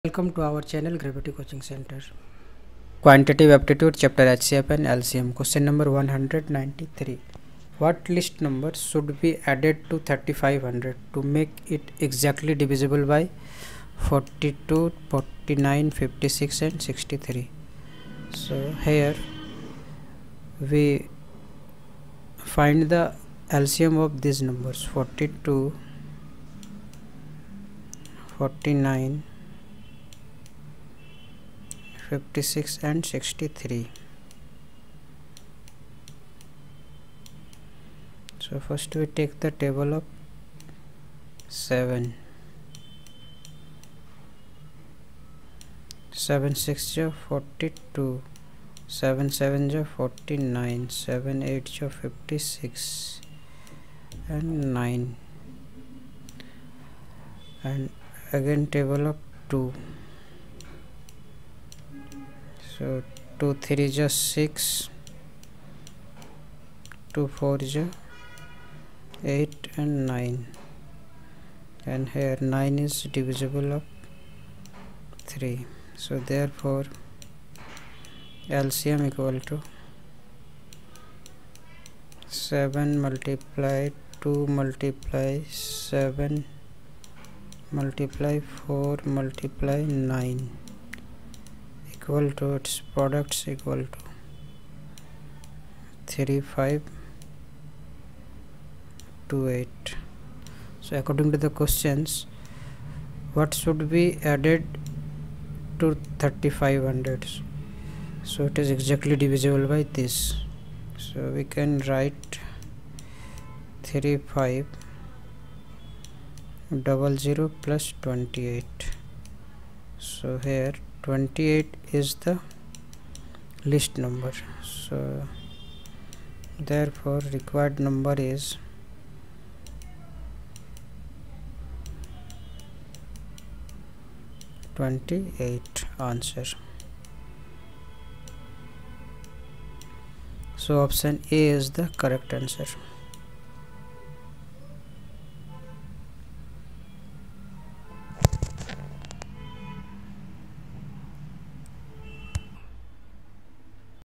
Q193 | What least number should be added to 3500 to make it exactly divisible by 42 49 56 and 63?
Click on below Links for more Questions:-
Qu(193):- What least number should be added to 3500 to make it exactly divisible by 42, 49, 56 and 63?